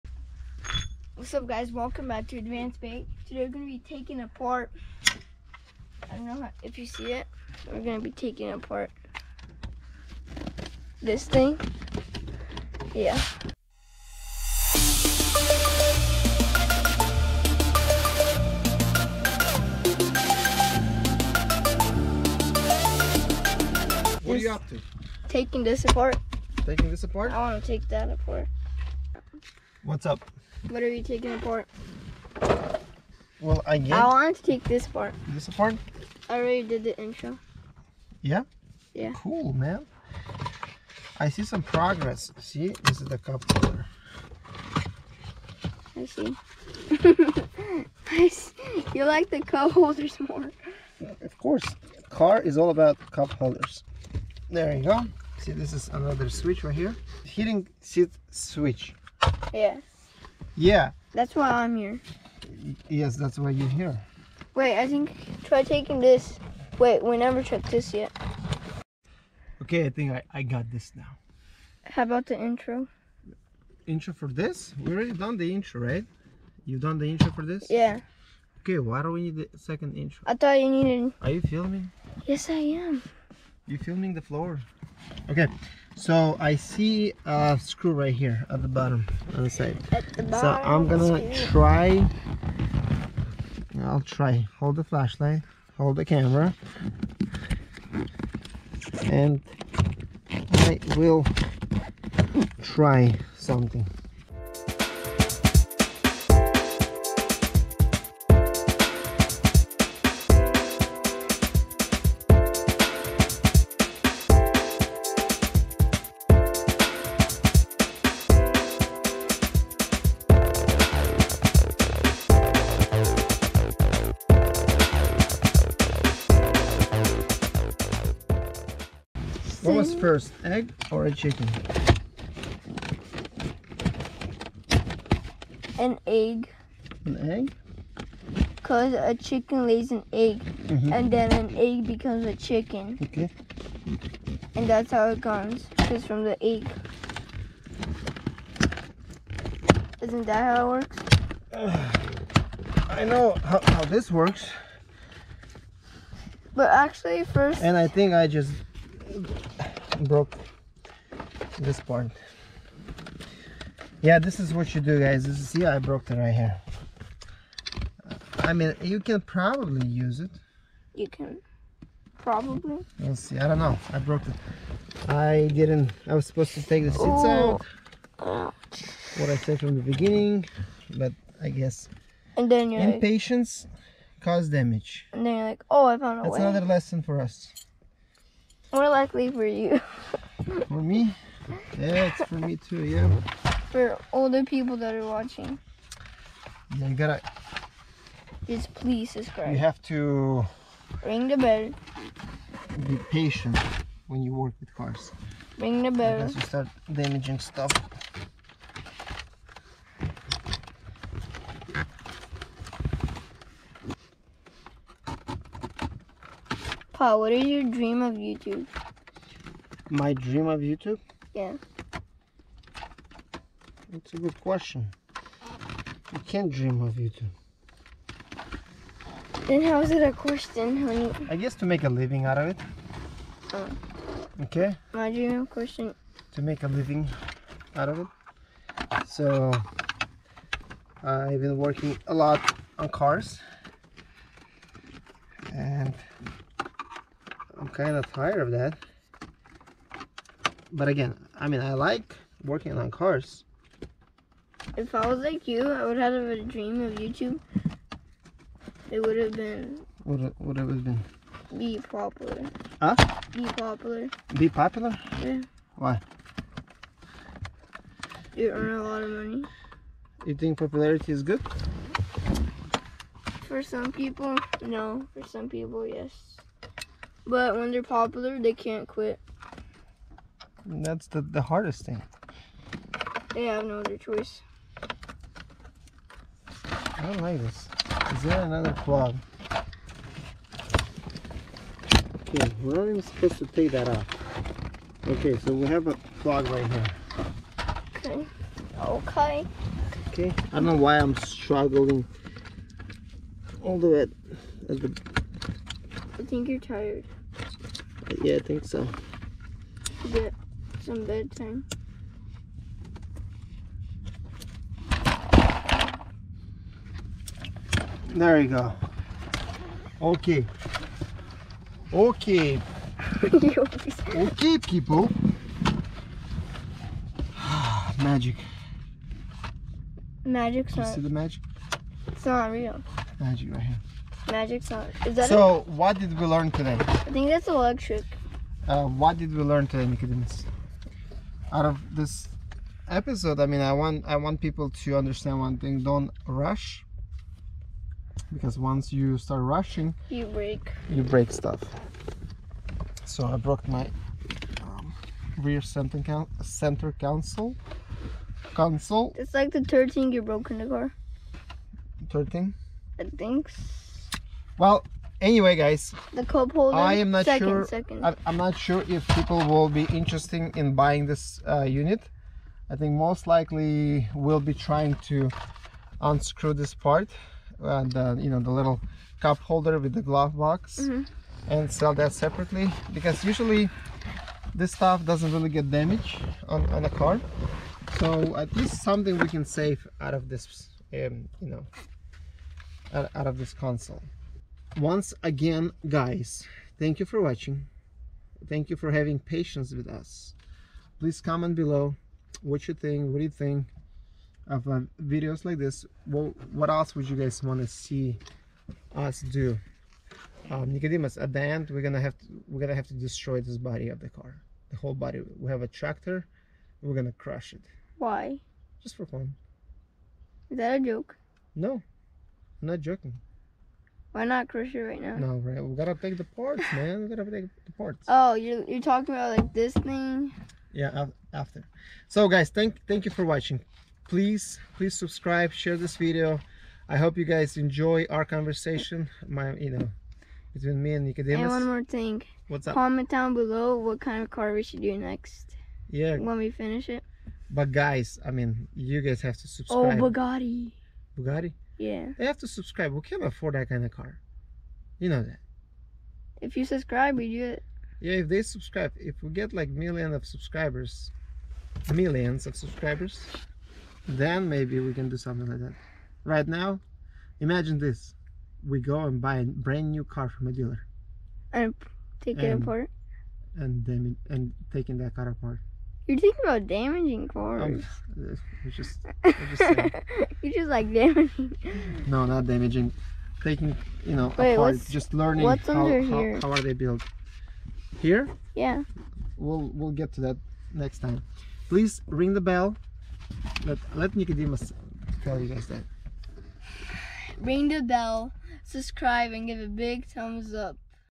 My 10 Year Old Son Taking Apart a Porsche Panamera 4S. How to Remove Rear Center Console
In this episode, I am teaching my 10-year-old son how to replace or remove the rear center console of a 2011 Porsche Panamera 4S.

Advancebay Inc. eBay Store
2011 Porsche Panamera 4S Parts For Sale.

Disassembly Porsche Panamera
Part 7 How to Replace or Remove Front Door Panel.